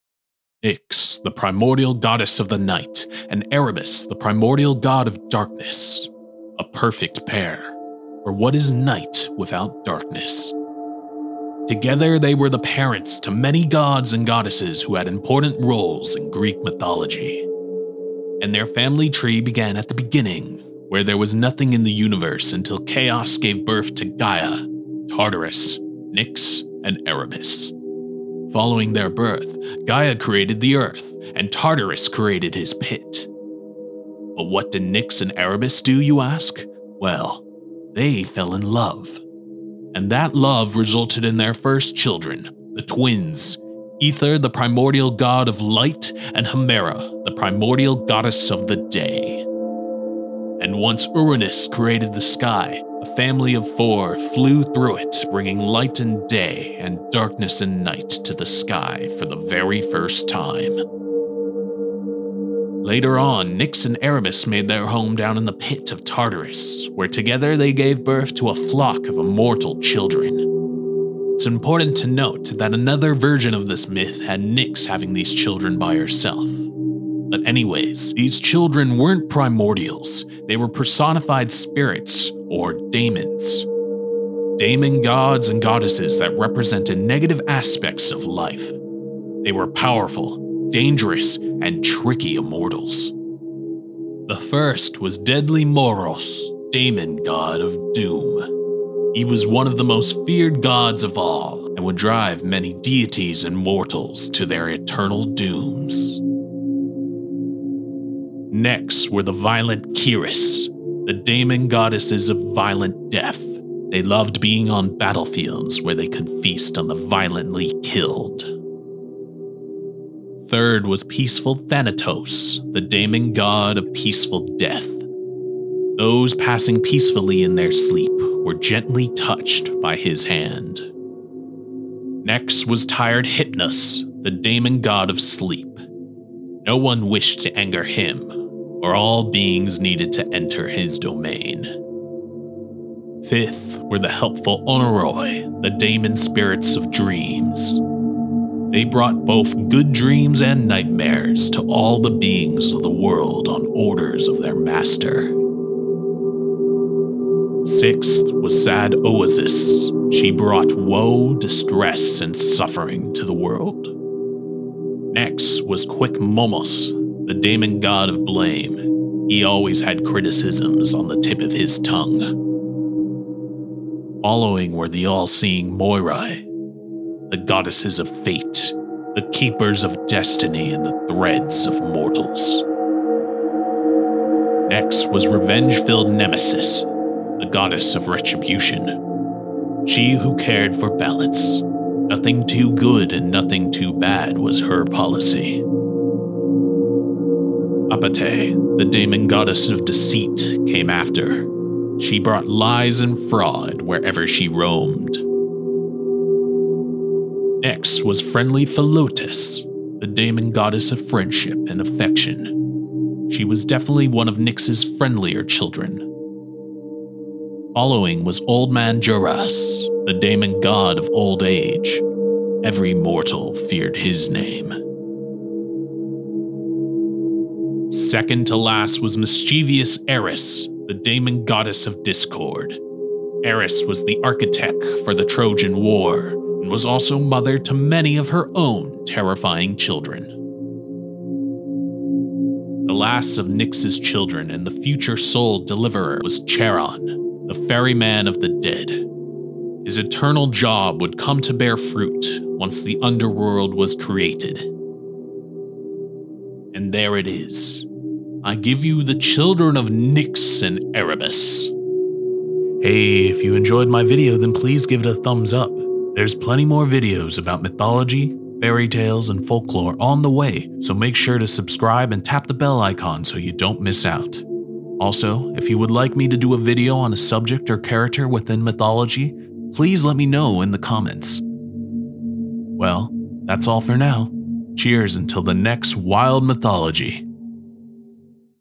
The Children of Nyx and Erebus - WILD Greek Mythology Family Tree
Today we explore the family tree of Nyx, the Primordial Goddess of Night and Erebus the Primordial God of Darkness from Greek Mythology. Come learn about their children including Eris the Goddess of Discord, Charon, the Ferryman, Thanatos the God of Death, Hypnos, the God of Sleep, and the Moirai the 3 Fates.